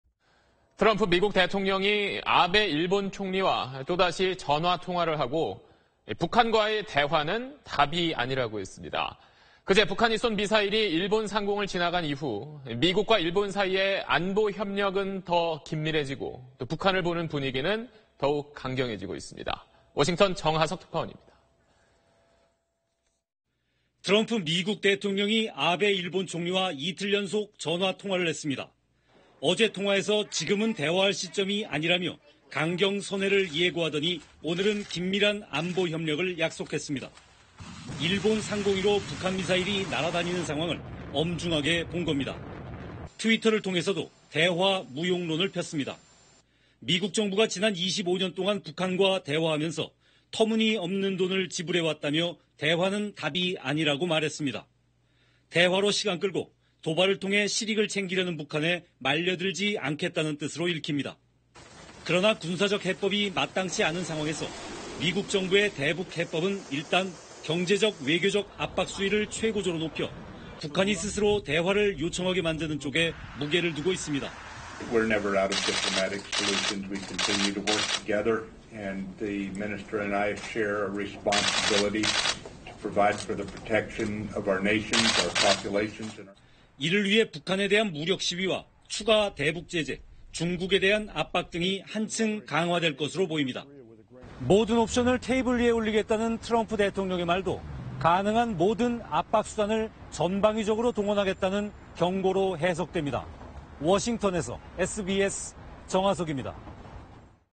"대화는 답 아냐" 트럼프-아베 통화 / SBS
트럼프 미국 대통령이 아베 일본 총리와 또다시 전화 통화를 하고 북한과 대화는 답이 아니라고 했습니다. 그제(29일) 북한이 쏜 미사일이 일본 상공을 지나간 이후 미국과 일본 사이의 안보 협력은 보다 긴밀해지고 북한을 보는 분위기는 더 강경해지고 있습니다.

워싱턴 정하석 특파원입니다.